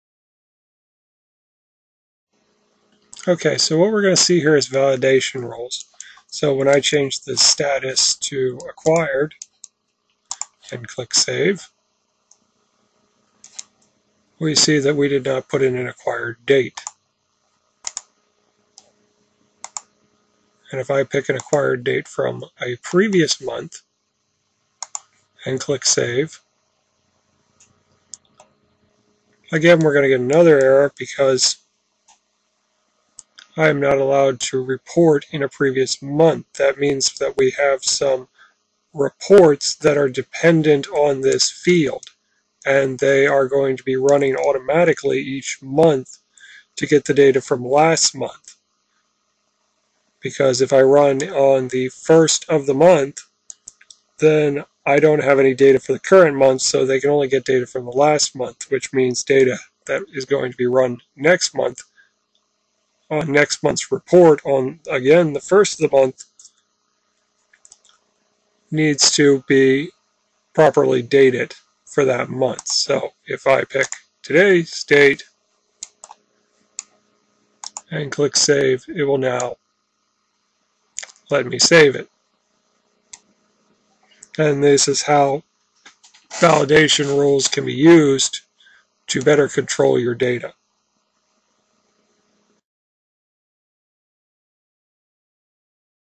salesforce validation rules demo
Demo of salesforce.com validation rules to control data entry. Also works on force.com . Validation rules work in professional edition and better.
In the demo validation rules are used to prevent a record from having date of before the curnet month because a report is to run on the first of each month and the amount earned or saved can not be reported on a report for a past month. It can only be recorded in current month or future date because the report for this month will run on the first day of next month.
to find out how we can help you.